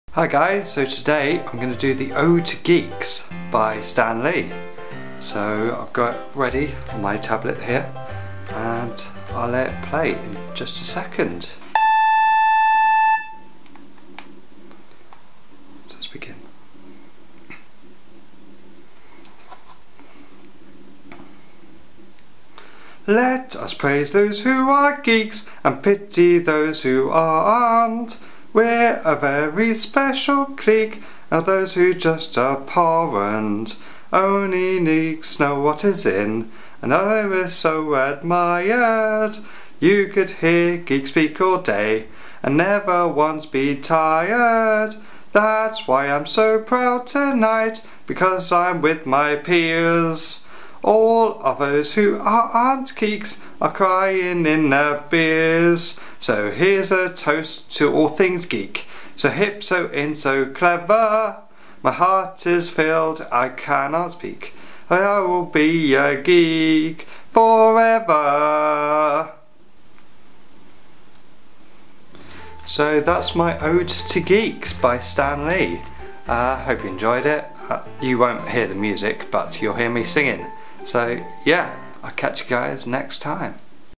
My Ode To Geeks - Stan Lee
I am Simon Reason and this is my Ode to Geeks by Stan Lee.